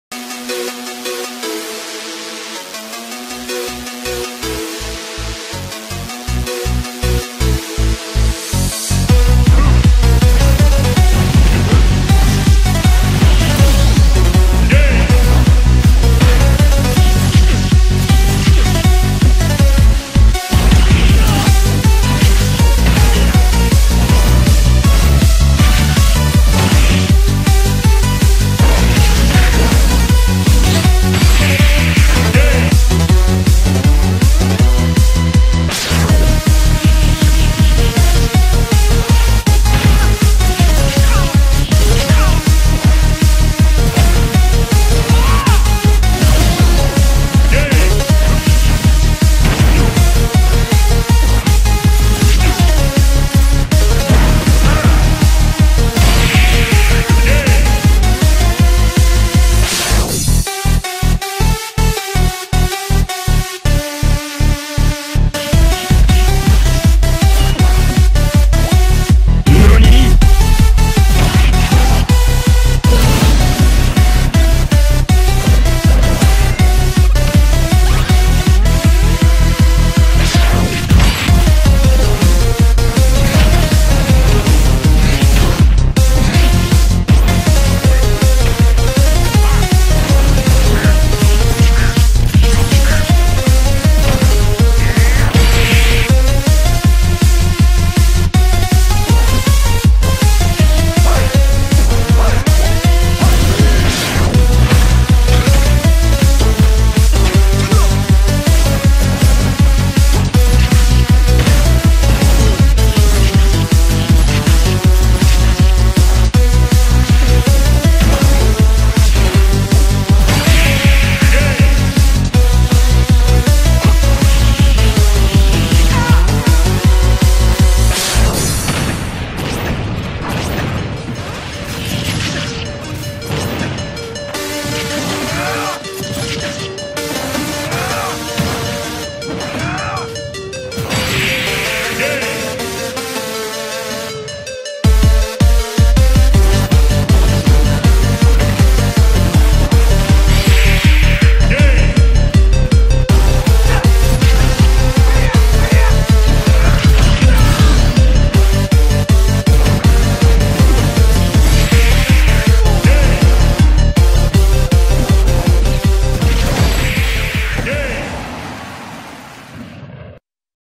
GB Maphoo "Everlasting Peace"
Second Montage on the channel Featuring GB Maphoo amaizng smash player and just an amazing addition to the team. Thank you all for the support and we hope to keep bringing the best content for you all!

Song made by Pixel Pirates

Edited by prophetofanger
His Twitter